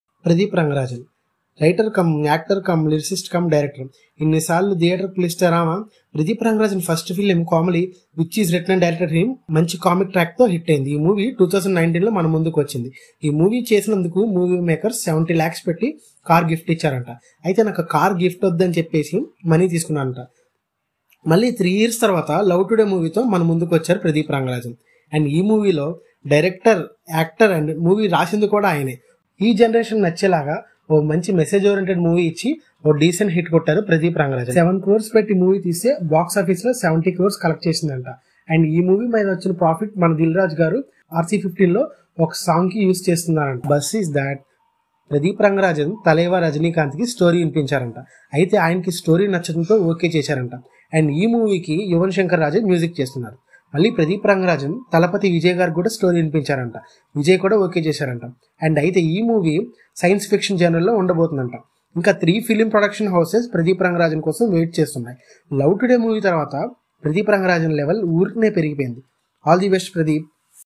pradeep ranganathan future plans next 2 projects with rajanikanth and thalapathy vijay | criticvu
Hello all and welcome to Criticvu. Hello all welcome tollyflix reviews
Pradeep ranganathan writer cum actor cum director cum lyricist.
The most successful director praddep ranganathan has came with his debut movie comali.After that he taken gap for 3 years and came with love today and given a decent blockbosrer.